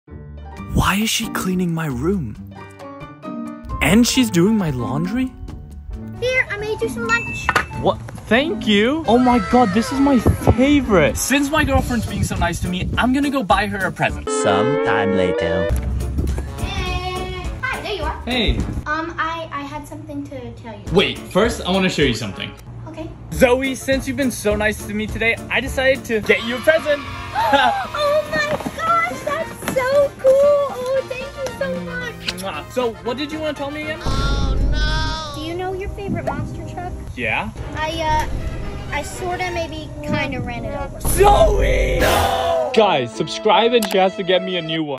She was LYING to me the WHOLE time... (Prank)
Zoe was being nice to me today, but I couldn't figure out why...